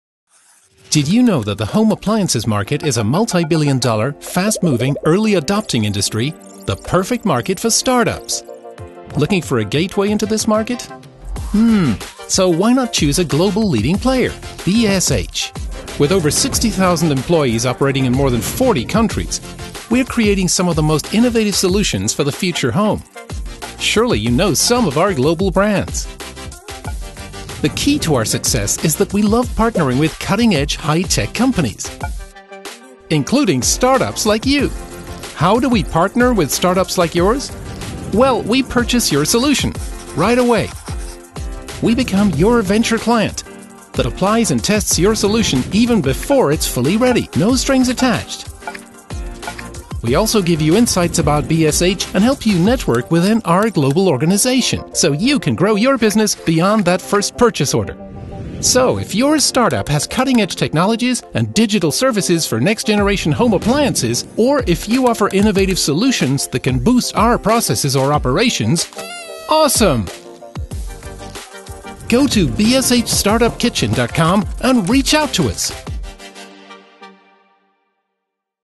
BSH StartUp Kitchen
We are excited to introduce our Venture Client Unit, the BSH Startup Kitchen! We offer a unique runway for startups into a revenue driven partnership with BSH - fast, flexible and no strings attached.